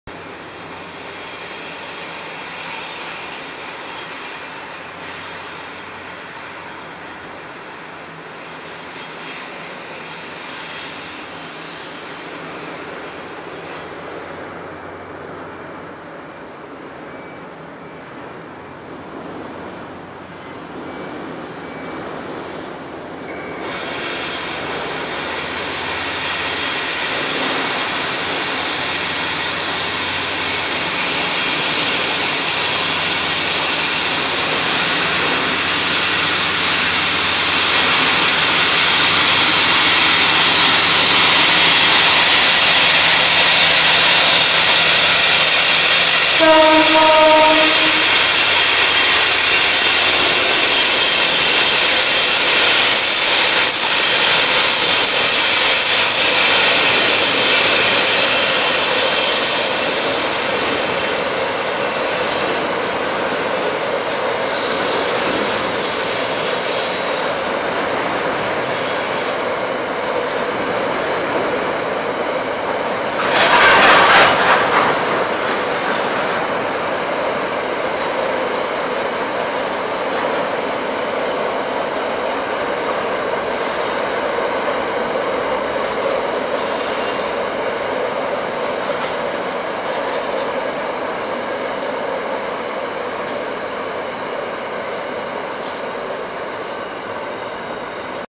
SCT Wine train with locomotives 1202 and 1201
The first time these locos have worked this service. Making their way back to Penfield over the Port River Bridge.